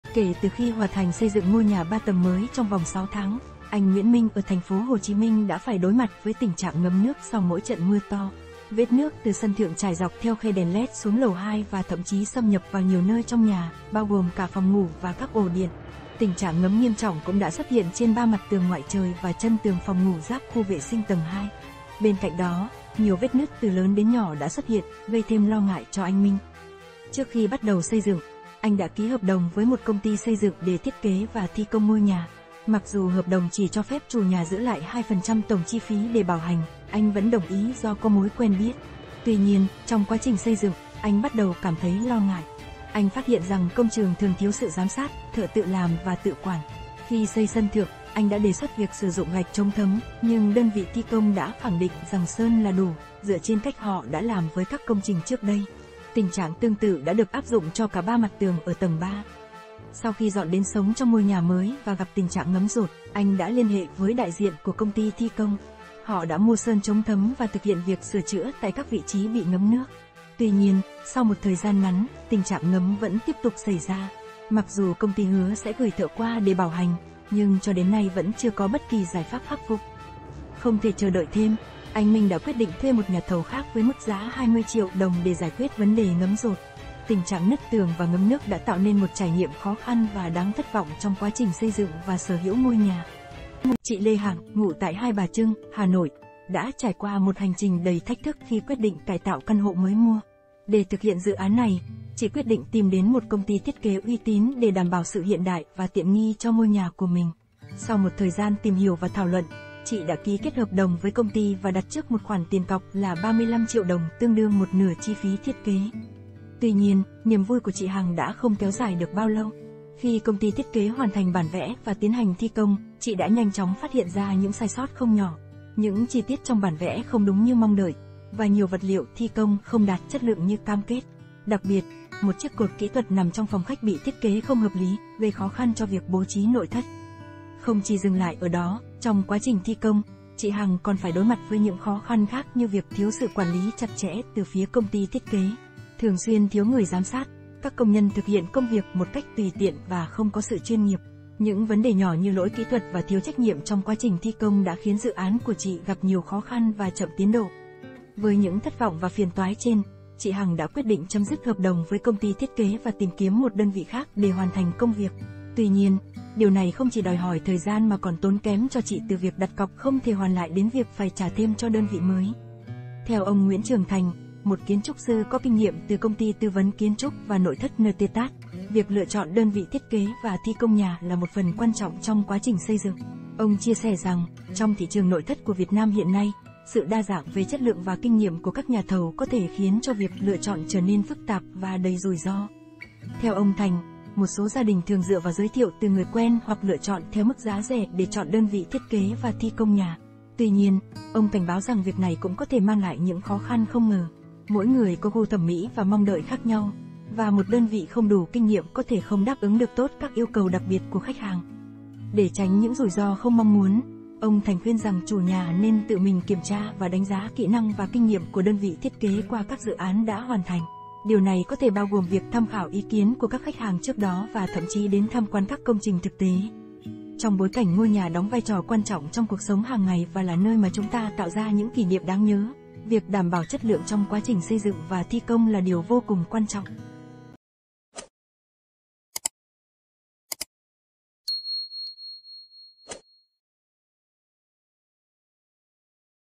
NGẤM DỘT VÀ VẾT NỨT TƯỜNG | CÂU CHUYỆN KHI XÂY NHÀ GẶP VẤN ĐỀ
Kể từ khi hoàn thành xây dựng ngôi nhà 3 tầng mới trong vòng 6 tháng, anh Nguyễn Minh ở TP  Hồ Chí Minh đã phải đối mặt với tình trạng ngấm nước sau mỗi trận mưa to. Vết nước từ sân thượng trải dọc theo khe đèn LED xuống lầu 2 và thậm chí xâm nhập vào nhiều nơi trong nhà, bao gồm cả phòng ngủ và các ổ điện. Tình trạng ngấm nghiêm trọng cũng đã xuất hiện trên 3 mặt tường ngoại trời và chân tường phòng ngủ giáp khu vệ sinh tầng 2. Bên cạnh đó, nhiều vết nứt từ lớn đến nhỏ đã xuất hiện, gây thêm lo ngại cho anh Minh.